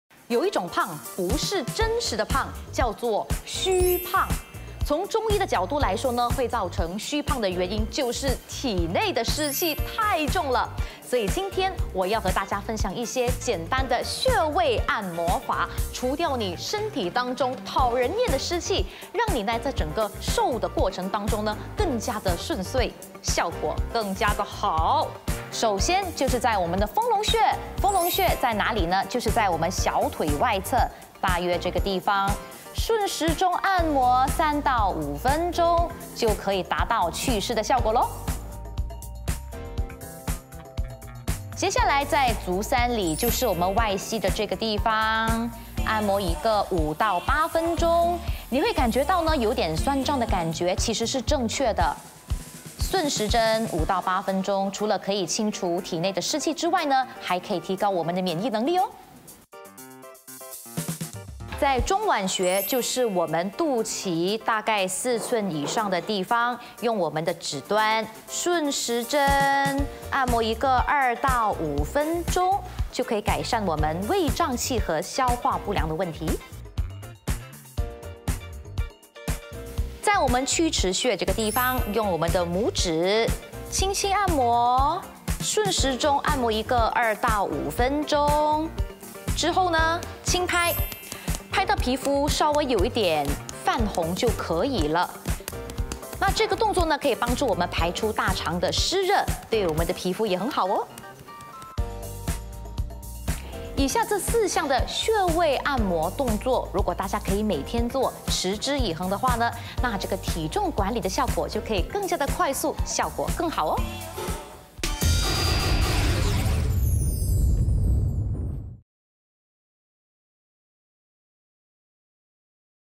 虚胖 - 消除湿气按摩 - 减肥法 | Go Fit with Champagne
虚胖，是亚健康所表现的人体状态之一，也是最常见的主流病症之一!
别怕有香槟在，教教大家按摩4个穴位调理哦~
简单的按摩一下自己的穴位，去除讨人厌的湿气！
別忘了守著ASTRO 303頻道，密切留意香檳的健身過程！
每逢星期四 下午2:30
每逢星期六 中午12:00
不见不散~